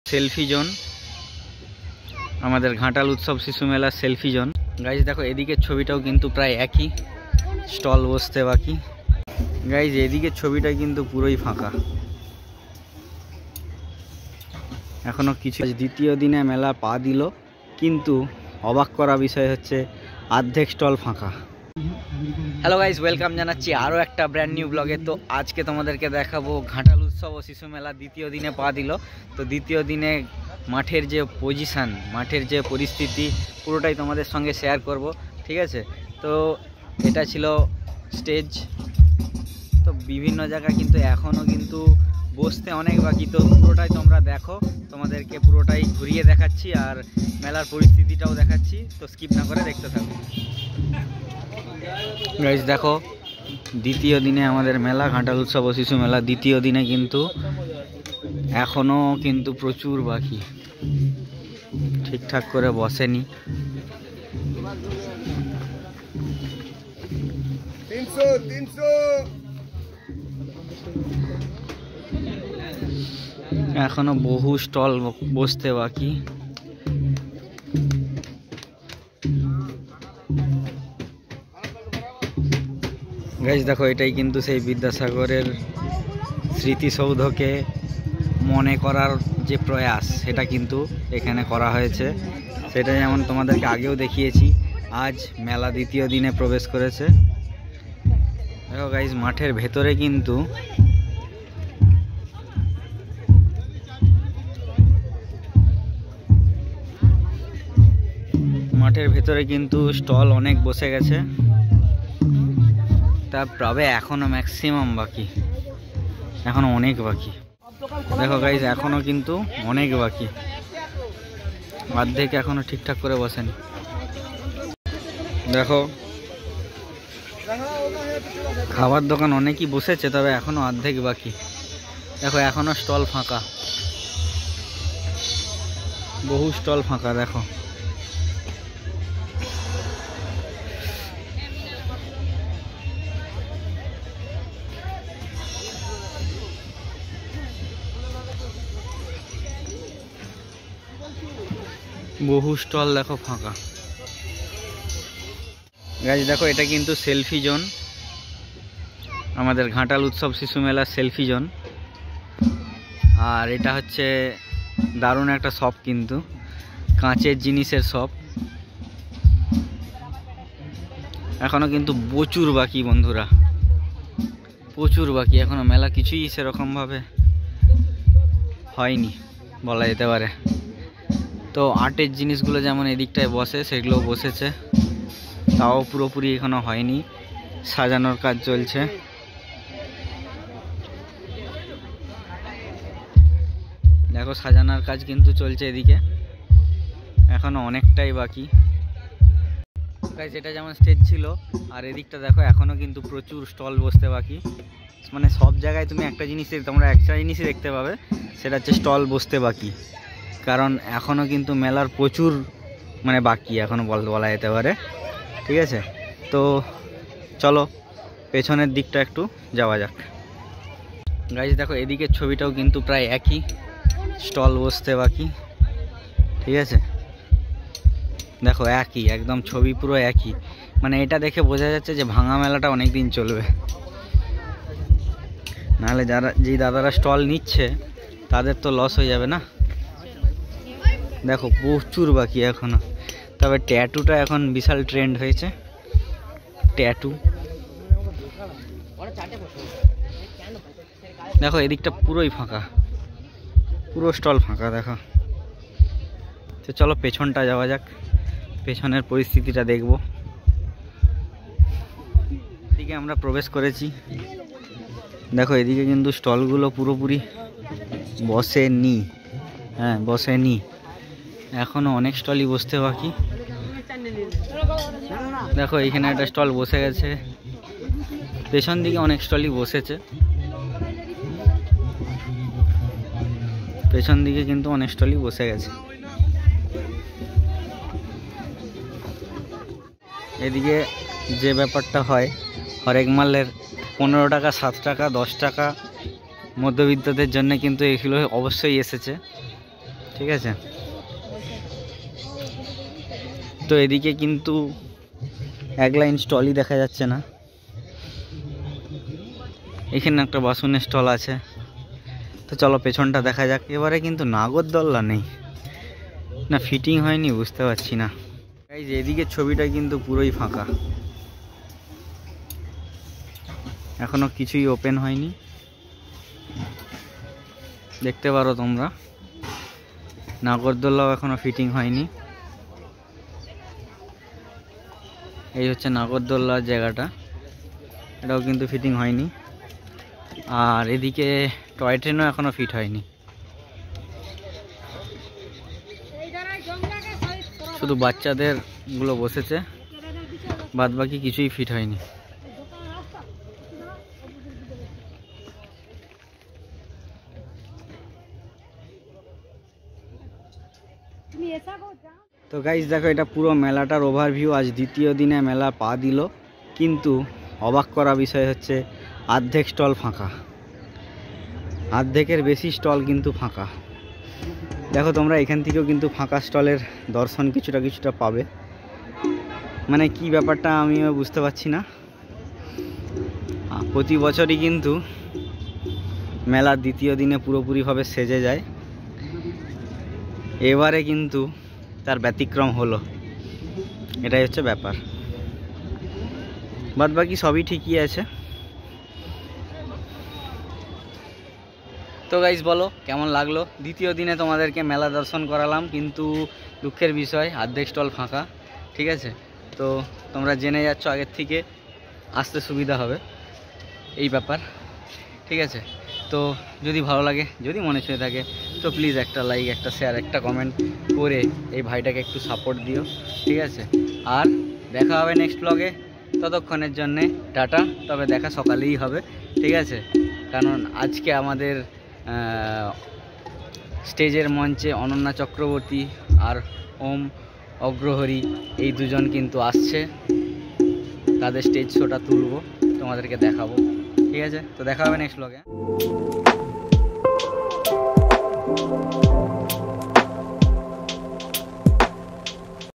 ঘাটাল উৎসব ও শিশু মেলা 2023||আজ মেলার দ্বিতীয় দিনে কিন্তু স্টল ফাঁকা অবাক করা বিষয়
মেলা দ্বিতীয় দিনে স্টল ফাঁকা অবাক বিষয়

# ঘাটাল#উৎসব#মেলা#২০২৩